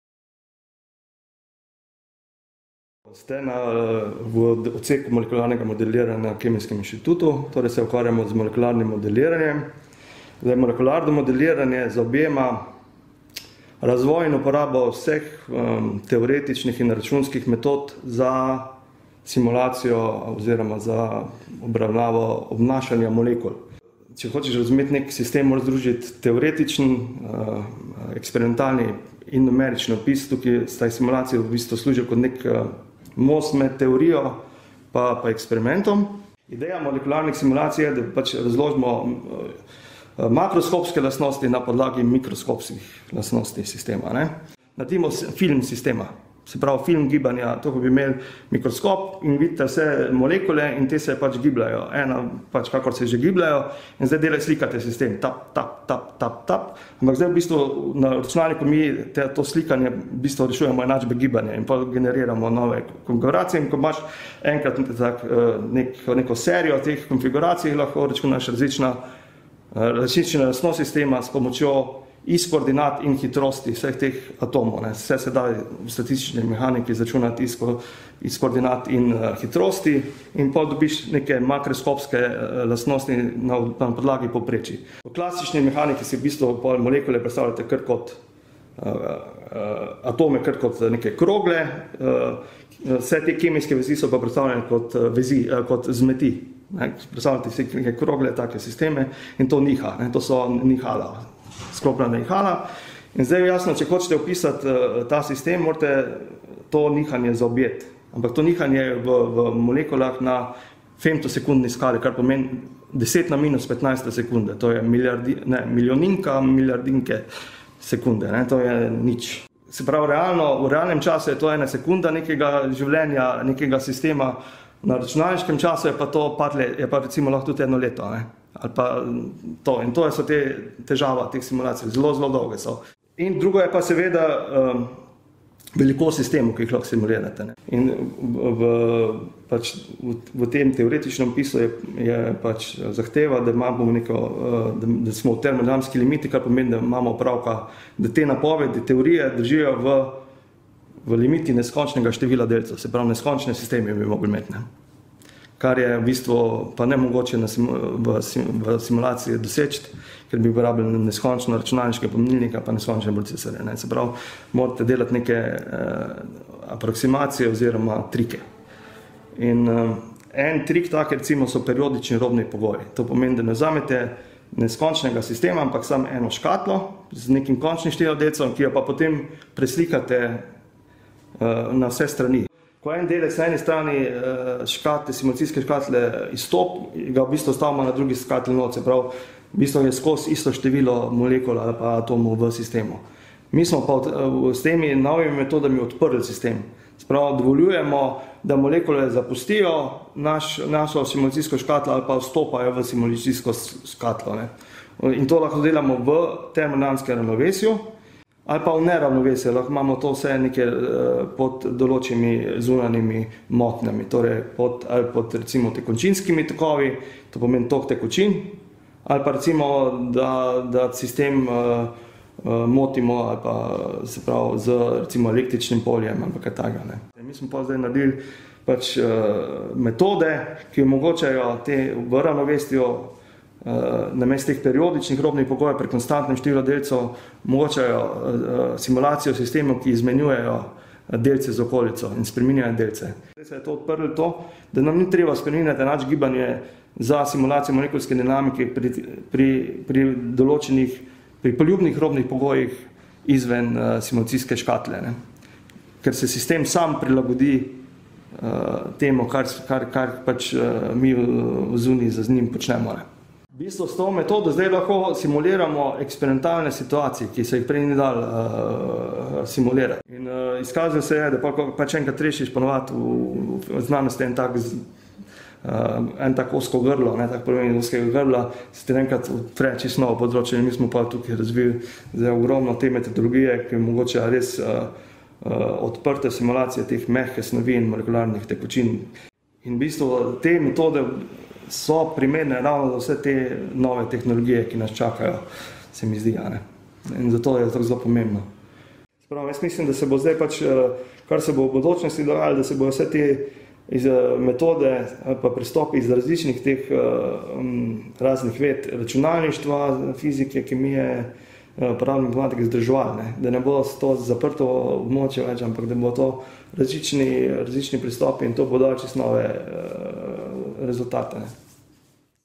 Znanstvenik Matej Praprotnik o dosežku s področja računalniških simulacij gibanja molekul
Znanstveniki Kemijskega inštituta so razvili nove računalniške simulacijske metode, ki bistveno razširjajo domet simulacij gibanja molekul v mehkih snoveh, kot so denimo tekoči kristali, polimeri in koloidi. Po besedah znanstvenika Mateja Praprotnika dosežek predstavlja premik na področju računalniških simulacij gibanja molekul, nove metode pa bi lahko imele pomembno vlogo pri razvoju nanotehnologij.